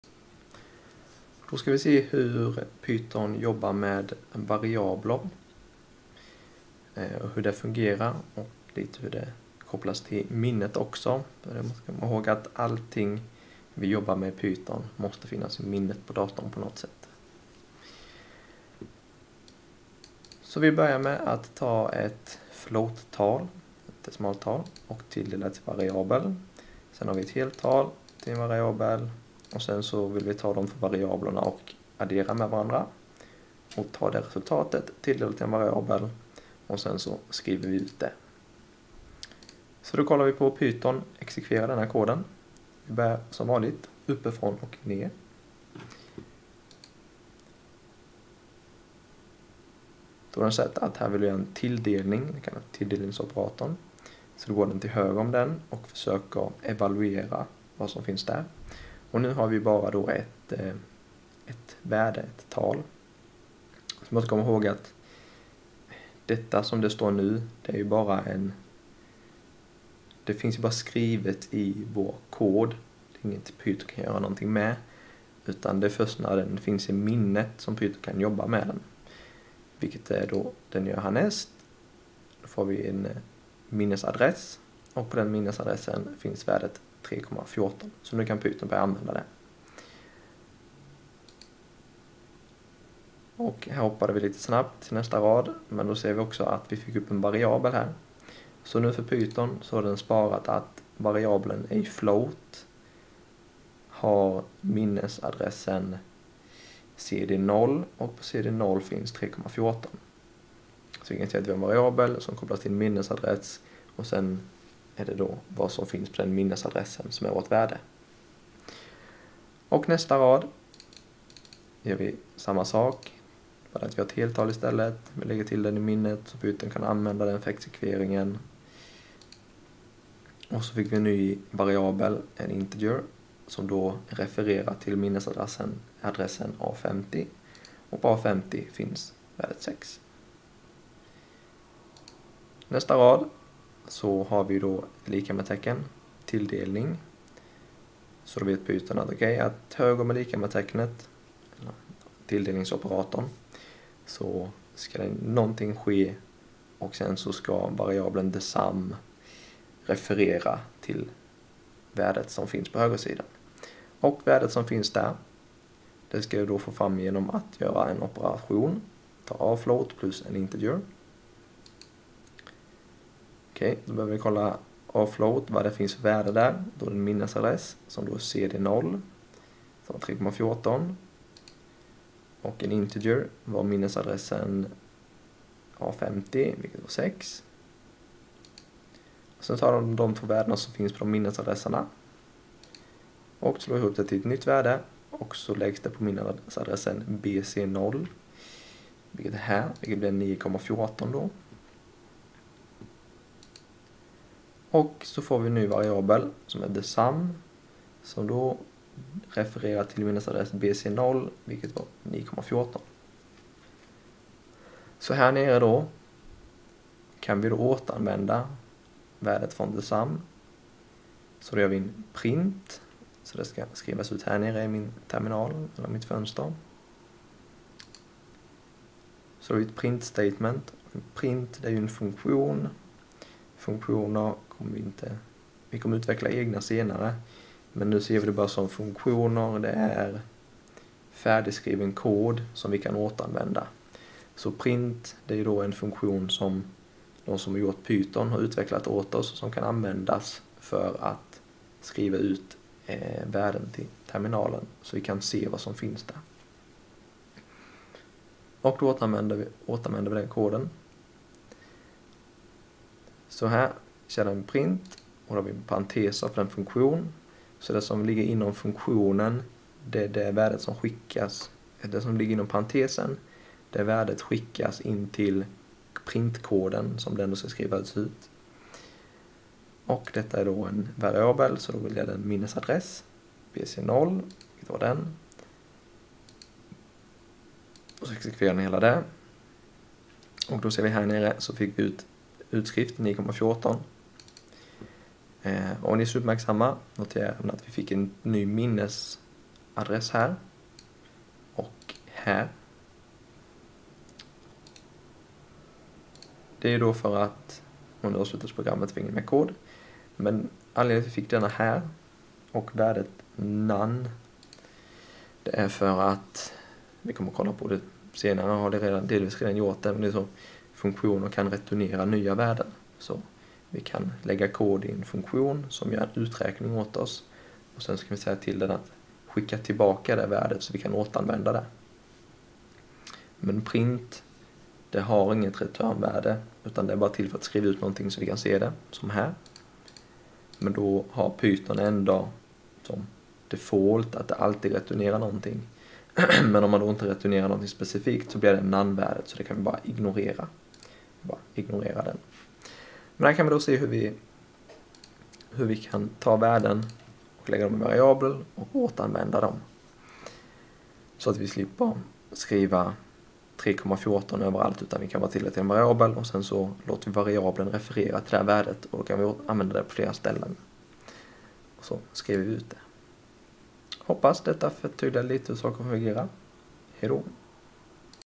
Hur variabler kopplas till värden i minnet
Andreas visar i vilken ordning python exekverar kod och spara värden i variabler.